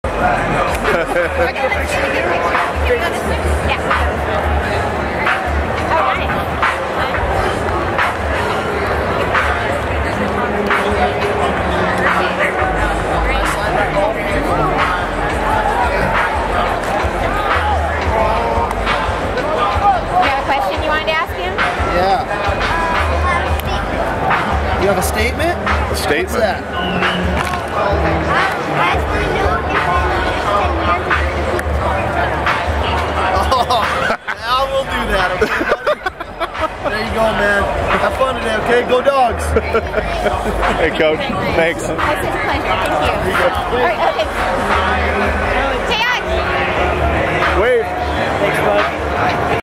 Alex Bradshaw and Coach Sark
Alex meets Huskies coach sark at the 2011 Washington vs. Colorado game